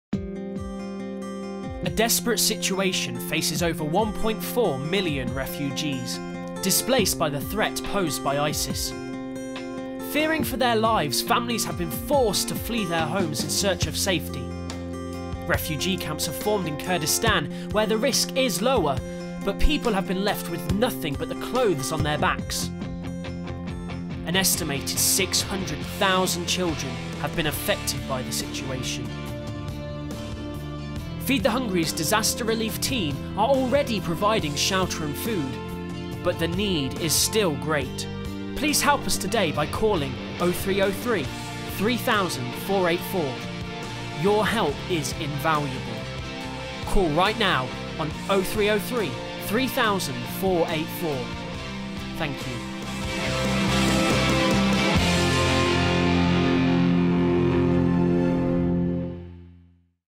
Feed the Hungry | Kurdistan Emergency Relief
As over 1.4 million refugees flee their homes to avoid the threat of ISIS, Feed the Hungry is stepping up to help.

We urgently need to send 100 tons of food and medical supplies to refugee camps in Kurdistan.

Please give what you can and help feed the hungry today.